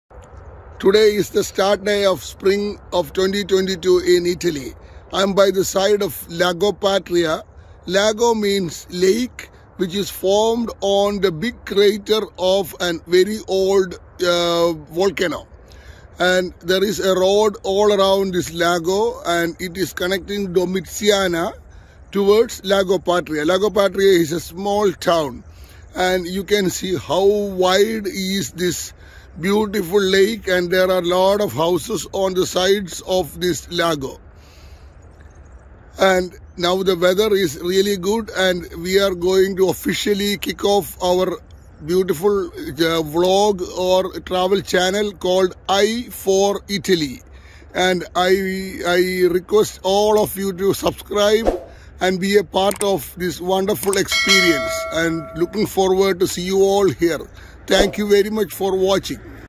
The beautiful Lago(Lake) Patria in Caserta, Naples in Italy
Lago Patria is a lake formed on the crater of a dormant volcano. It is very wide and beautiful. Several houses overlook the lago. A road goes around the Lago Patria all the way from Domiziana to Lago Patria town. This is the first video from I for Italy the travel vlog to show the life in italy.